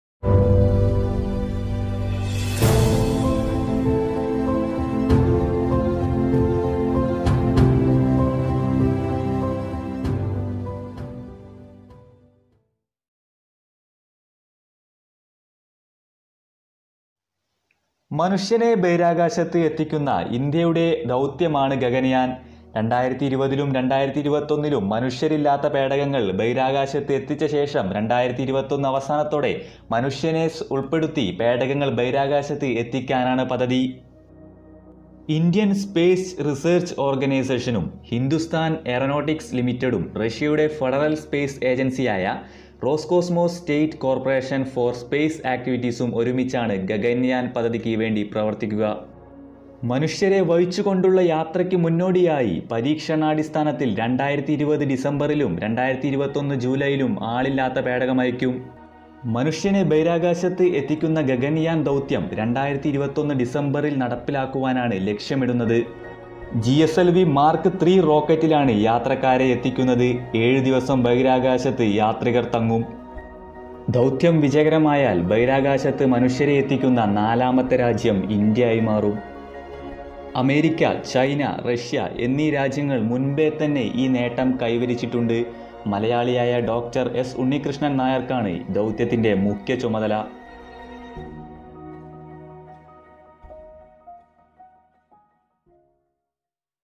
Gaganyaan mission Malayalam explained
Gaganyaan . Next space mission ISRO

gaganyaan spacecraft  program

Gaganyaan is an Indian crewed orbital spacecraft intended to be the basis of the Indian Human Spaceflight Programme. The spacecraft is being designed to carry three people, and a planned upgraded version will be equipped with rendezvous and docking capability

No copyrighted contents are used in this video